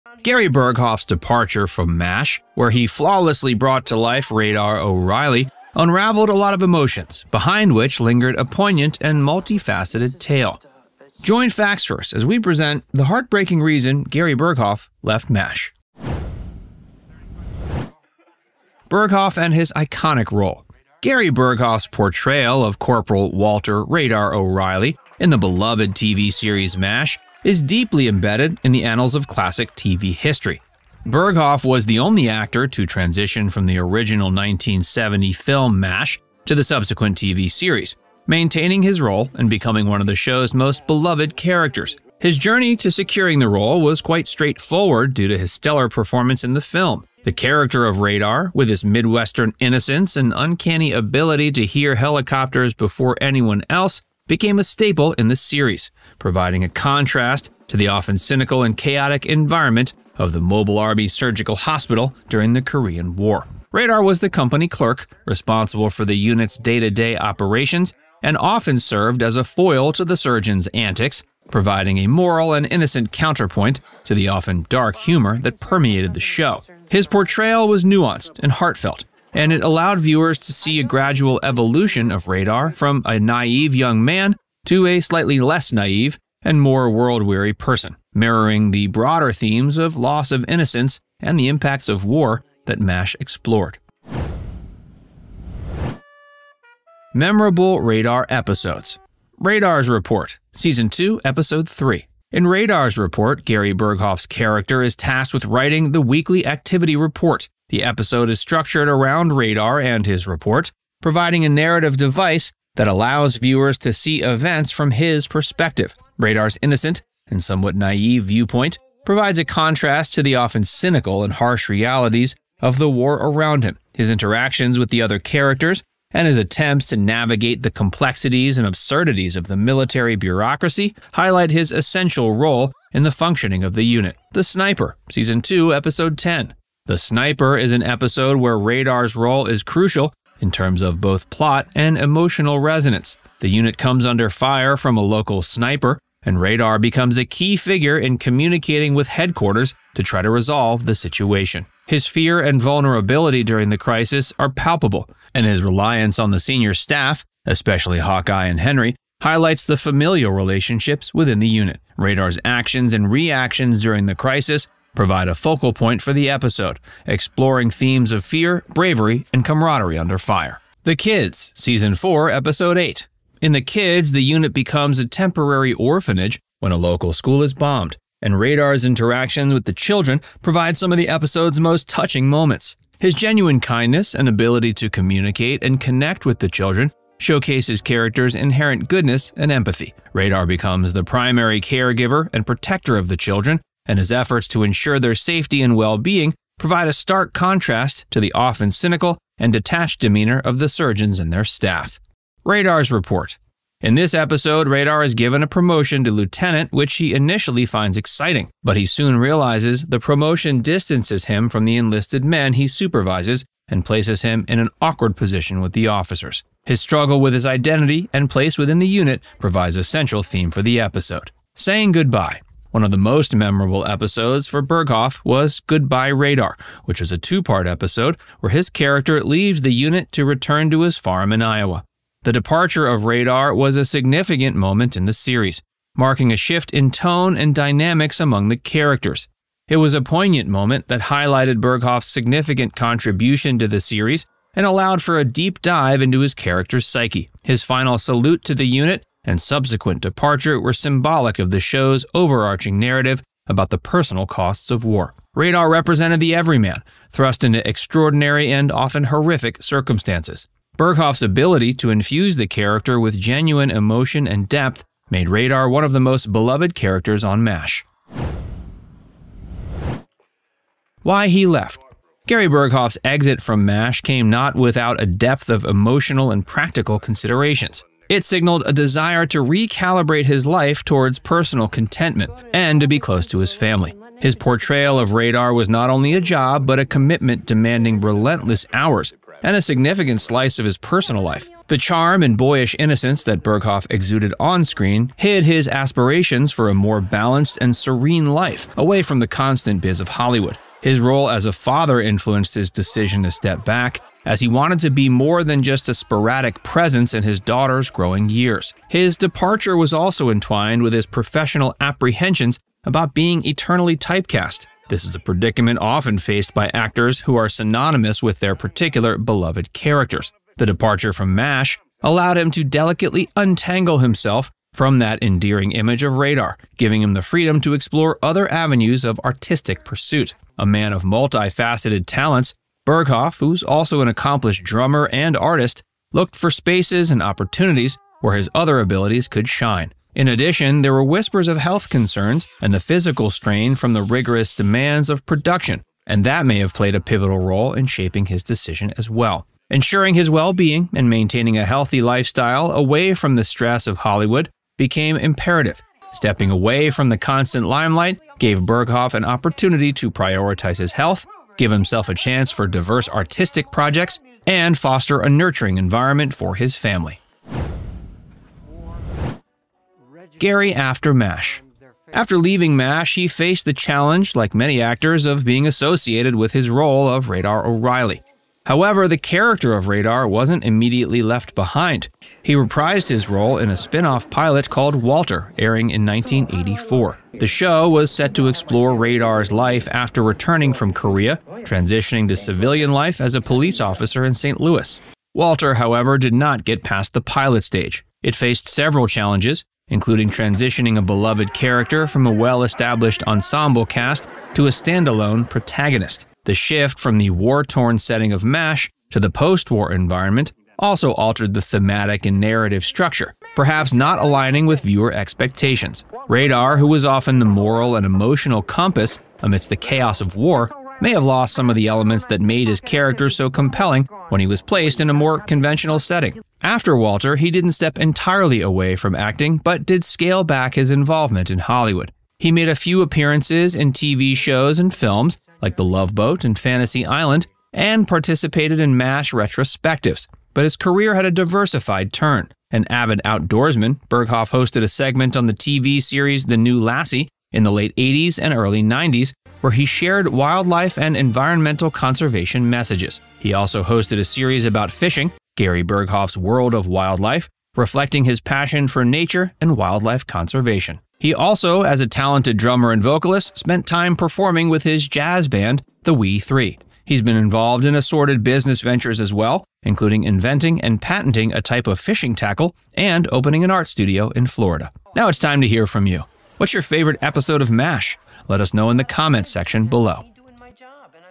The Heartbreaking Reason Gary Burghoff Left MASH
The entrancing cadence of Gary Burghoff's portrayal of Radar O'Reilly in the seminal series "M*A*S*H" rendered him a beloved figure, adored by fans for his impeccable blend of endearing naivety and surprising profundity. His departure from the series harbored under its surface a tapestry of heartbreaking motivations, weaving together both personal and professional strands that pulled Burghoff away from the iconic 4077th Mobile Army Surgical Hospital.

00:00 - Intro
00:18 - Gary Burghoff and His Iconic Role in MASH
01:36 - Memorable Radar Episodes
04:30 - Why He Left
06:10 - Gary Burghoff After MASH
07:55 - Outro

The seemingly incongruent image of the cherished actor, who with deft precision conveyed a youthful, optimistic Corporal, stepping away from the spotlight was, in reality, a poignant reflection of Burghoff’s own grappling with the relentless pressures and sacrifices that accompanied his fame. Exhaustion, both physical and emotional, frayed at his being as the taxing schedule and perpetual public scrutiny began overshadowing the joys of acting. Amidst the applause and acclaim, Burghoff nursed a longing for a life that permitted the flourishing of personal relationships and afforded the tranquility away from the incessant hum of Hollywood.

His young daughter, and a desire to be present as she navigated through her tender years, beckoned him towards a path that prioritized paternal love over professional pursuits. Furthermore, Burghoff, an artist at heart, sought to explore avenues that enabled him to express his creativity beyond Radar’s uniform, unfettered by the typecasting that often fettered actors to their most recognizable roles. His departure, thereby, stood as a resonant, albeit heartrending, testament to the often-unseen sacrifices made by those in the public eye, embodying a narrative where the pursuit of personal fulfillment and familial bonds gently nudged a beloved actor away from the warm, albeit sometimes harsh, glow of the spotlight, and into the comforting embrace of a life less scrutinized, yet richly woven with intimate, invaluable moments. Join Facts Verse, as we present: The Heartbreaking Reason Gary Burghoff Left M*A*S*H